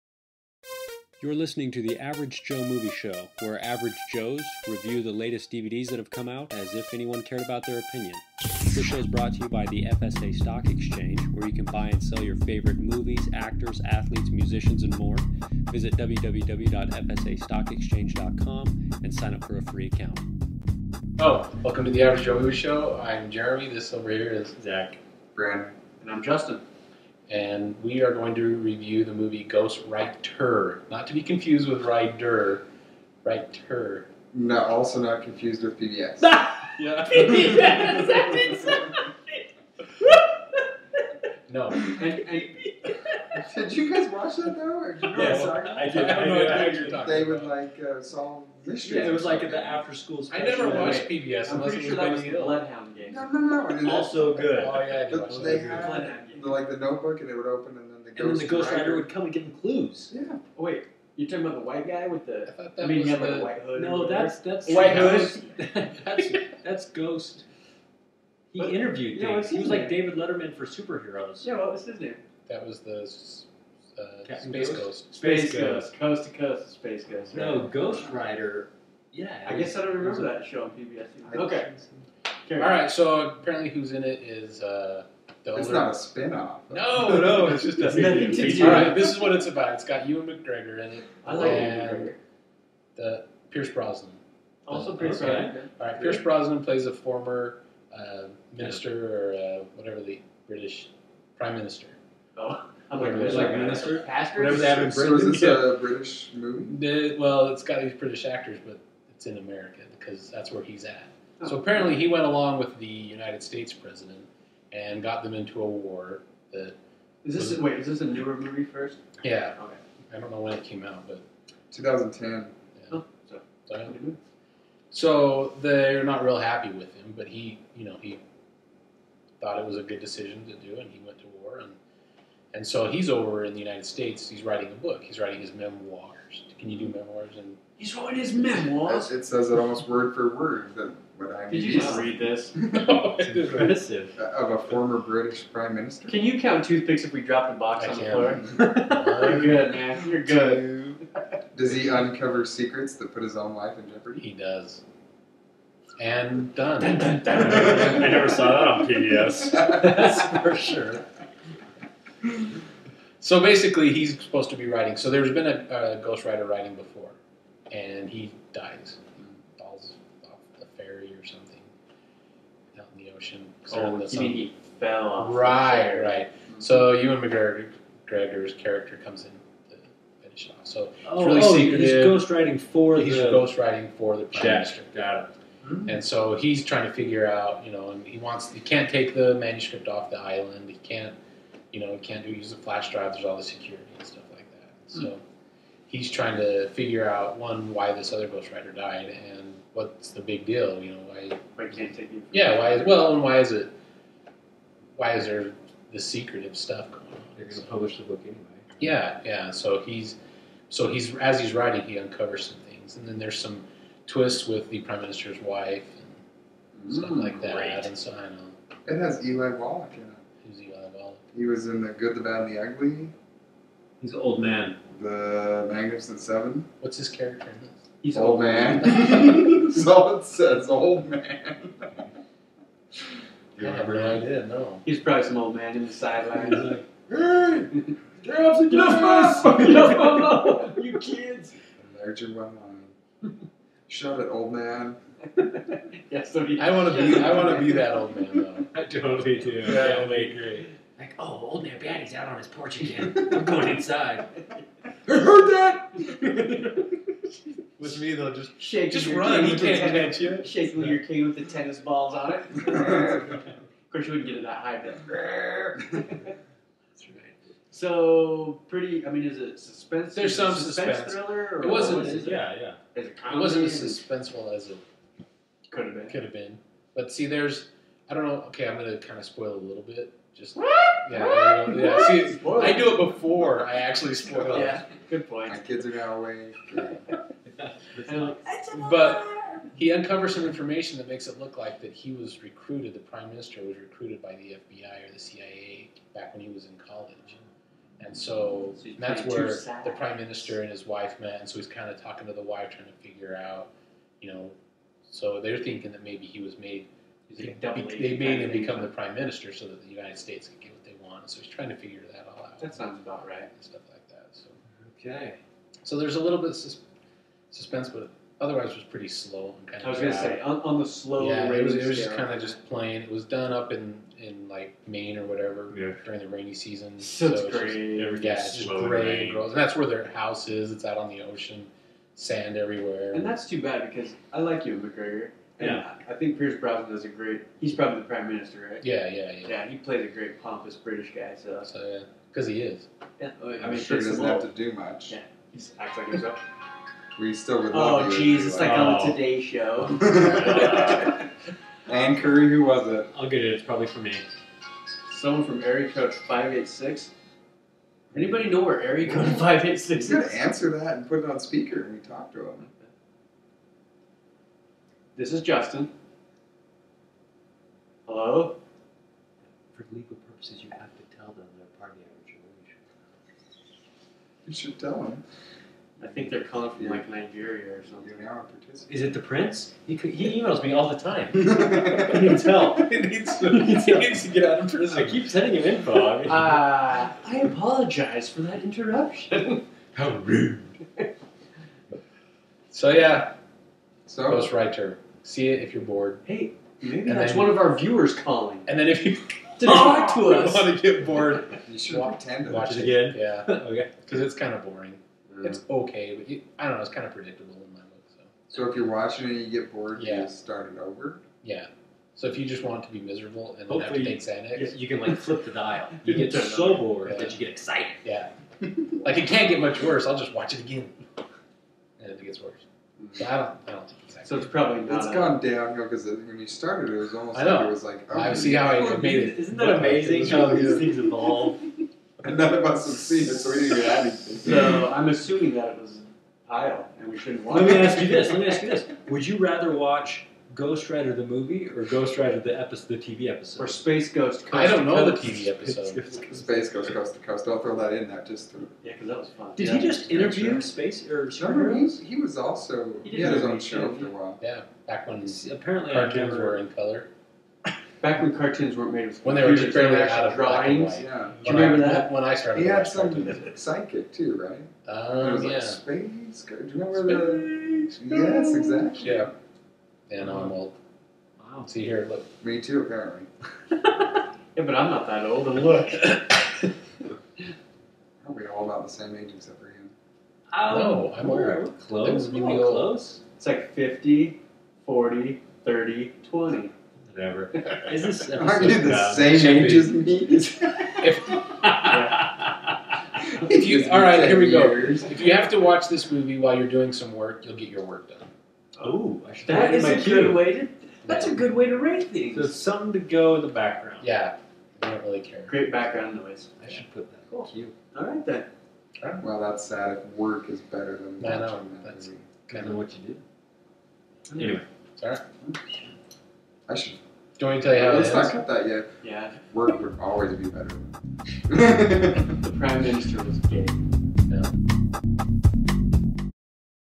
AJMS 34: Ghost Writer
The Average Joe Movie Show.

The Joes review Ghost Writer, starring Ewan McGregor
Jon Bernthal
Tim Preece
James Belushi
Timothy Hutton
Anna Botting
Yvonne Tomlinson
Milton Welsh
Alister Mazzotti
Tim Faraday
Kim Cattrall
Kate Copeland
Soogi Kang
Lee Hong Thay
Olivia Williams